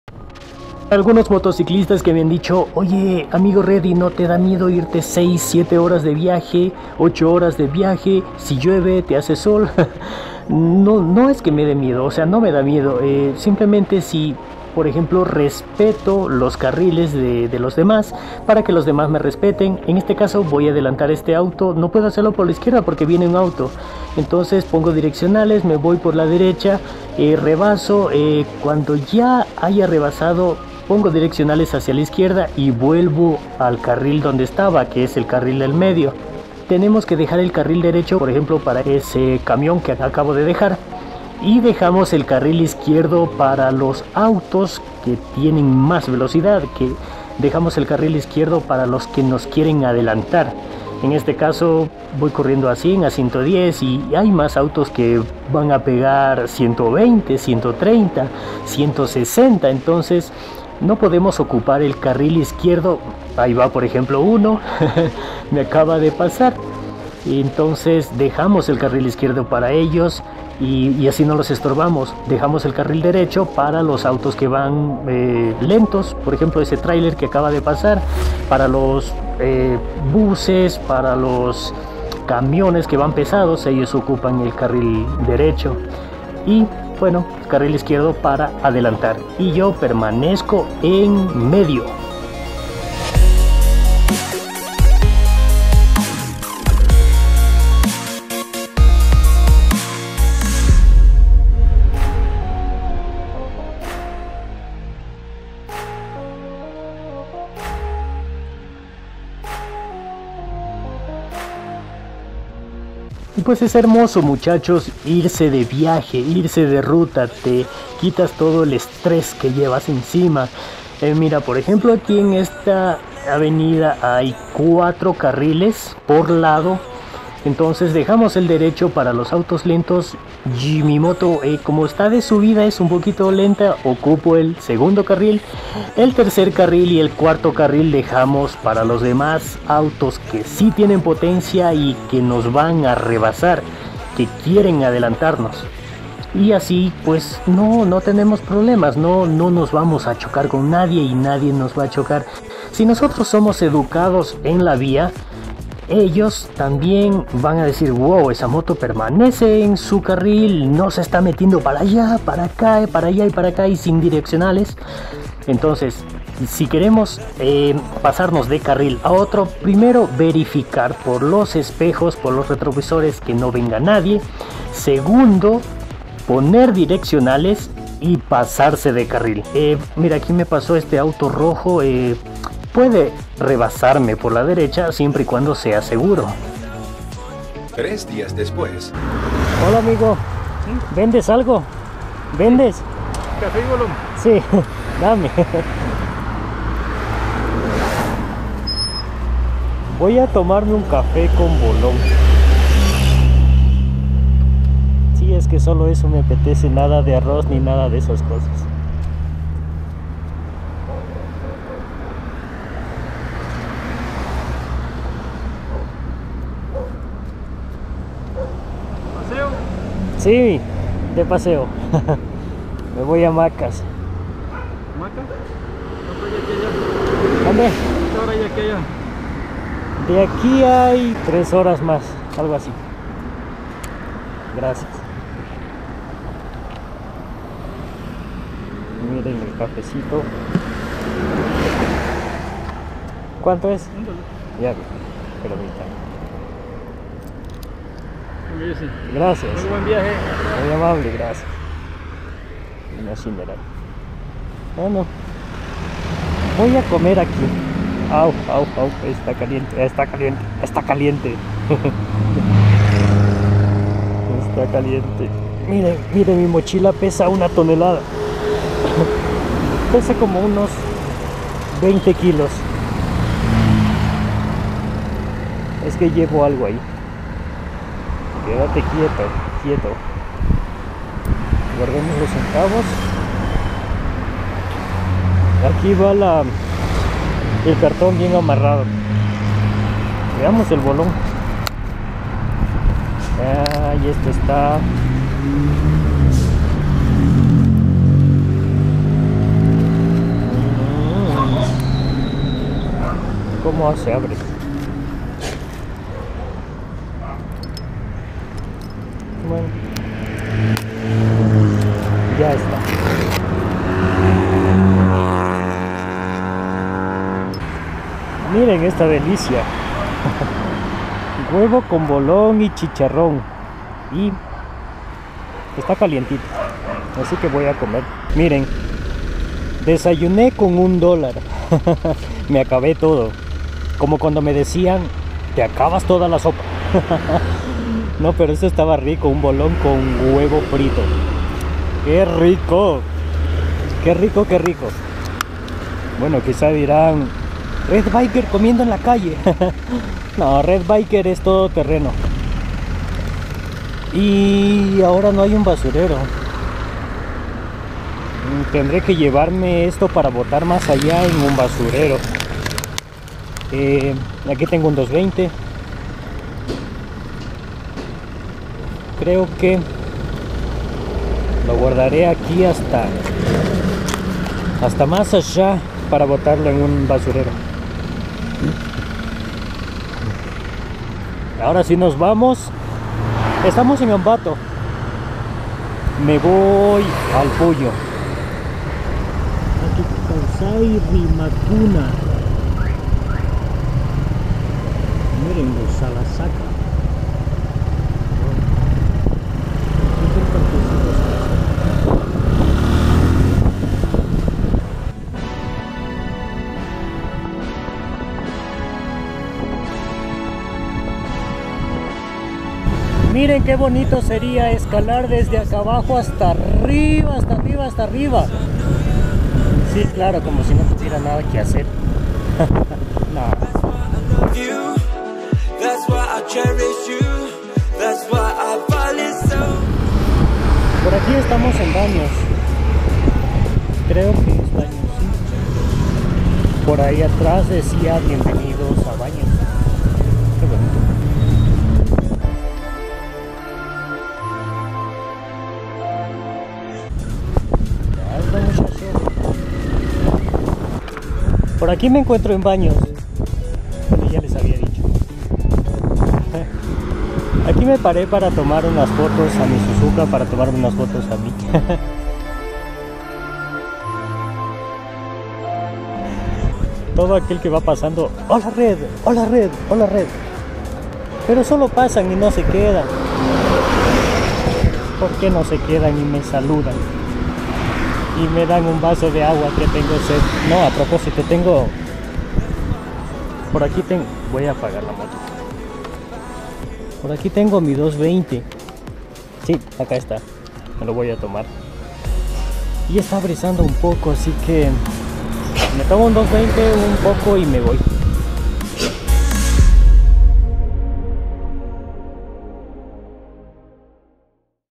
Viaje en Moto desde Quito a Morona Santiago parte 2_ conduciendo en Autopista_ desayune con 1 dólar
Motos de cros
Viajando en moto desde la ciudad de Quito hasta la ciudad de Macas
 PARTE 2
me han preguntado como conduzco una moto 6 hasta 8 horas
aquí les dejo cómo conducir una moto en avenidas y carreteras en autopista paso a paso
la importancia de mirar los retrovisores, antes de cambiarte de carril debes poner direccionales, la importancia de permanecer en un solo carril de acorde a la velocidad en la que vas en tu moto
aparte de eso me detuve a desayunar en la vereda alado de mi moto y también pase por Ambato, pase por Pelileo y me detuve en Baños de agua santa